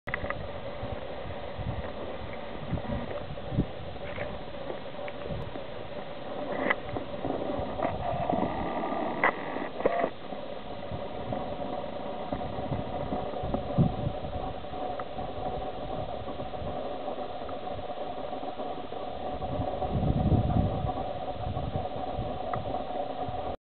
Vent & Turban Geysers
A pair of geysers near Old Faithful in Yellowstone